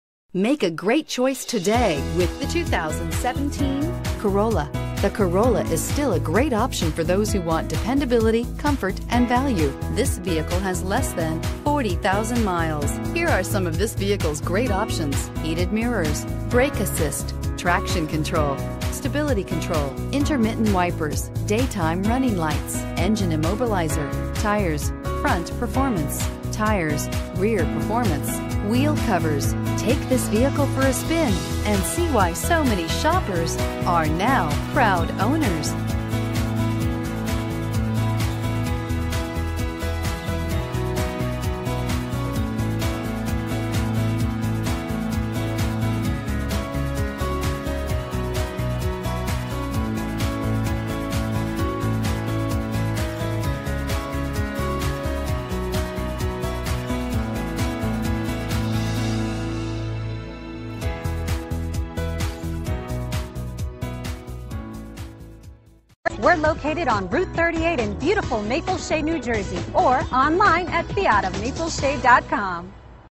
2017 Toyota Corolla Cherry Hill NJ FAIR-805
2017 Toyota Corolla LE

IIHS Top Safety Pick+. Boasts 36 Highway MPG and 28 City MPG!